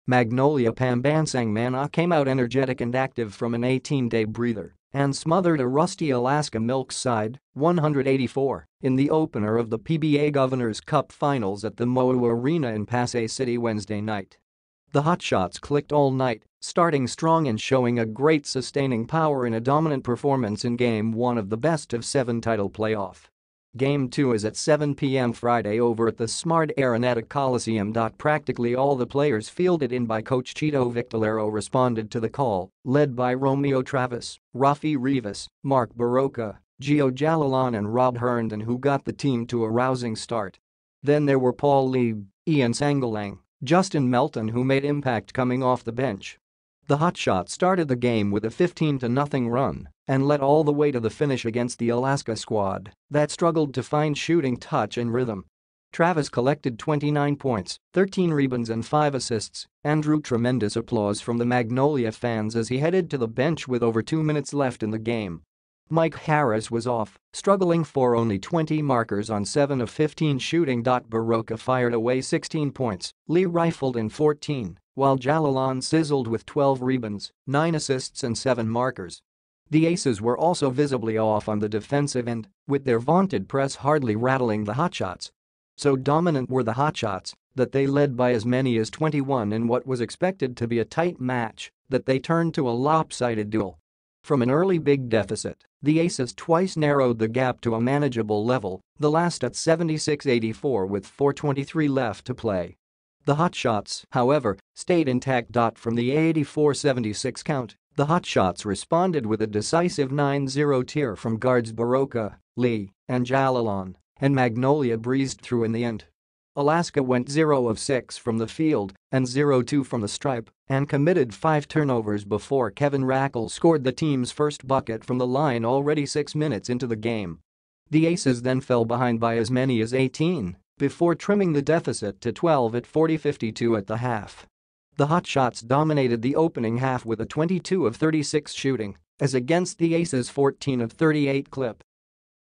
Sizzling Hotshots torch Aces in series opener
Magnolia Pambansang Manok came out energetic and active from an 18-day breather and smothered a rusty Alaska Milk side, 100-84, in the opener of the PBA Governors Cup Finals at the MOA Arena in Pasay City Wednesday night.

The Hotshots clicked all night, starting strong and showing a great sustaining power in a dominant performance in Game One of the best-of-seven title playoff.

Game Two is at 7 p.m. Friday over at the Smart Araneta Coliseum.

Practically all the players fielded in by coach Chito Victolero responded to the call, led by Romeo Travis, Rafi Reavis, Mark Barroca, Jio Jalalon and Rob Herndon who got the team to a rousing start.

Then there were Paul Lee, Ian Sangalang, Justin Melton who made impact coming off the bench.

The Hotshots started the game with a 15-to-nothing run and led all the way to the finish against the Alaska squad that struggled to find shooting touch and rhythm.

Travis collected 29 points, 13 rebounds and five assists and drew tremendous applause from the Magnolia fans as he headed to the bench with over two minutes left in the game.

Mike Harris was off, struggling for only 20 markers on 7-of-15 shooting.

Barroca fired away 16 points, Lee rifled in 14 while Jalalon sizzled with 12 rebounds, nine assists and seven markers.

The Aces were also visibly off on the defensive end, with their vaunted press hardly rattling the Hotshots.

So dominant were the Hotshots that they led by as many as 21 in what was expected to be a tight match that they turned to a lopsided duel.

From an early big deficit, the Aces twice narrowed the gap to a manageable level, the last at 76-84 with 4:23 left to play. The Hotshots, however, stayed intact.

From the 84-76 count, the Hotshots responded with a decisive 9-0 tear from guards Barroca, Lee and Jalalon, and Magnolia breezed through in the end.

Alaska went 0-of-6 from the field and 0-2 from the stripe, and committed five turnovers before Kevin Racal scored the team's first bucket from the line already six minutes into the game.

The Aces then fell behind by as many as 18 before trimming the deficit to 12 at 40-52 at the half.

The Hotshots dominated the opening half with a 22-of-36 shooting as against the Aces' 14-of-38 clip.